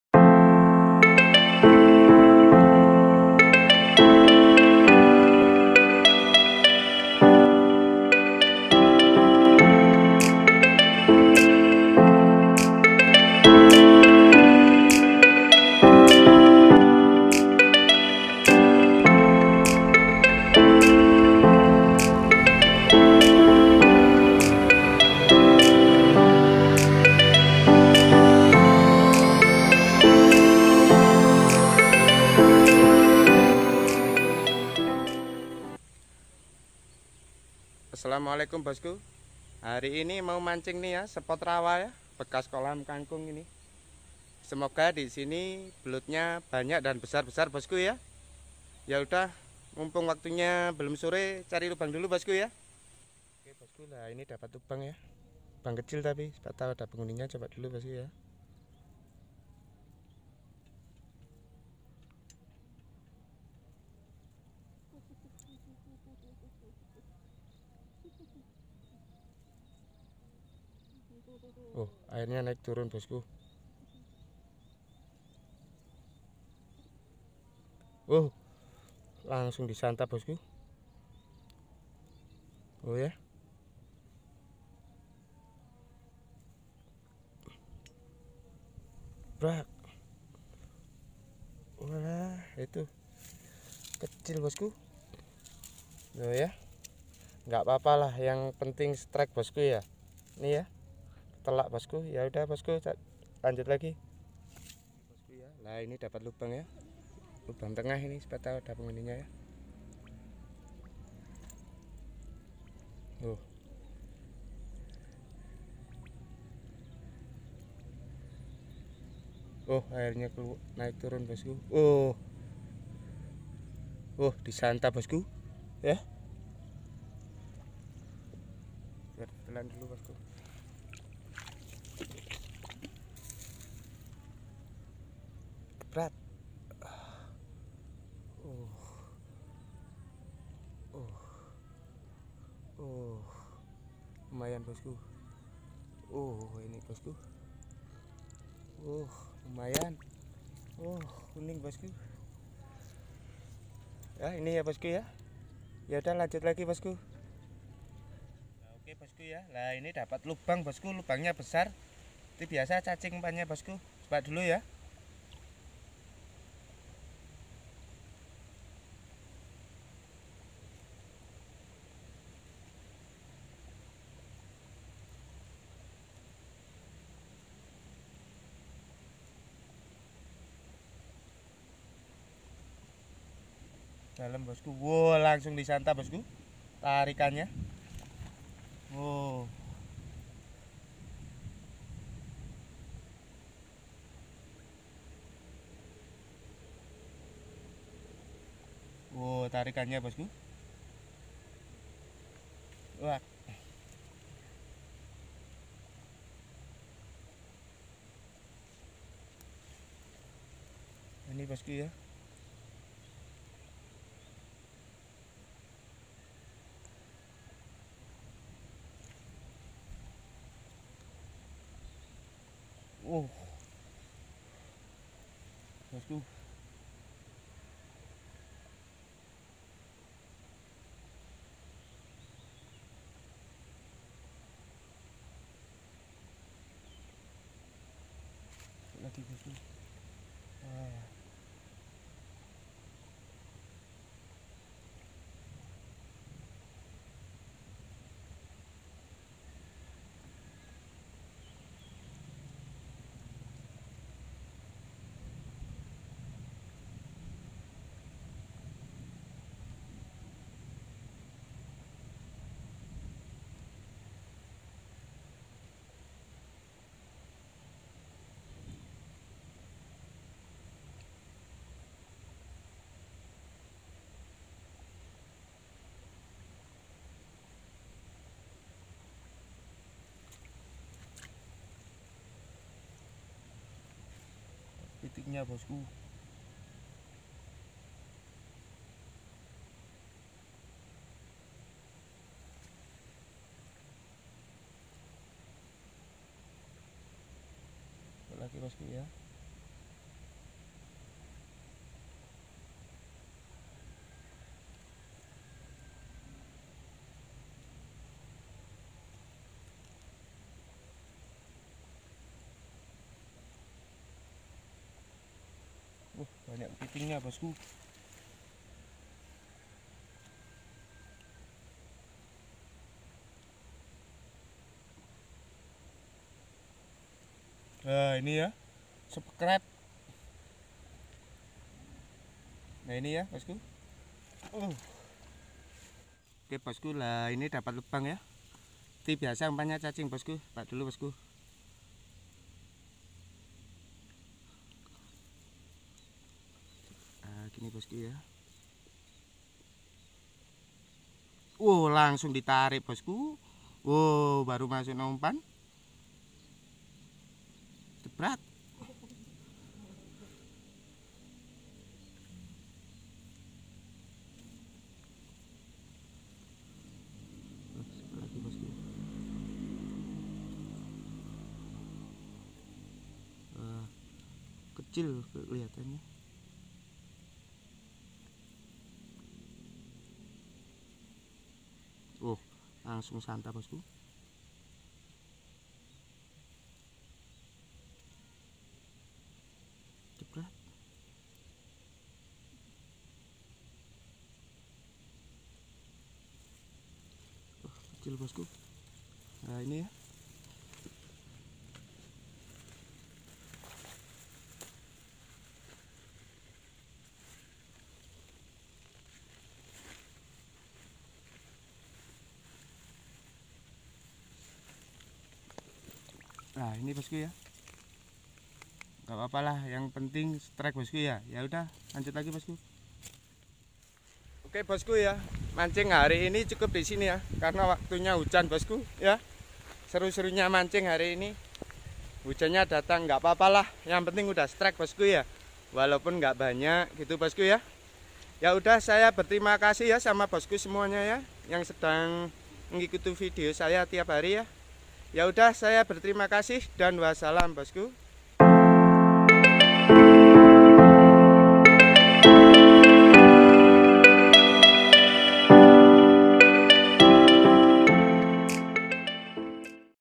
Mancing belut di spot Singosari Malang hasilnya mengejutkan..?
Asalammualaikum bos ku...

Kesempatan kali ini saya mancing belut  di spot rawa tanaman kangkung sangat seru baget  bosku semoga video saya kali ini dapat menghibur bos ku

Terima kasih sudah melihat video saya jangan lupa

Wasalammualaikum Wr - Wb